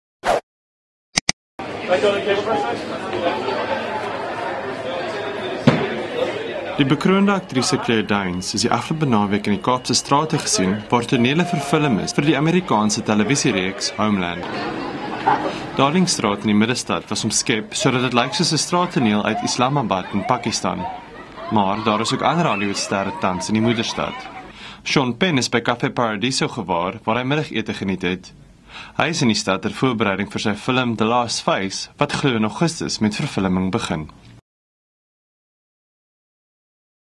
Hollywood sterre in Kaap
Die bekroonde aktrise Claire Danes is die afgelope naweek in die Kaapse strate gesien waar tonele verfilm vir die Amerikaanse televisiereeks, Homeland.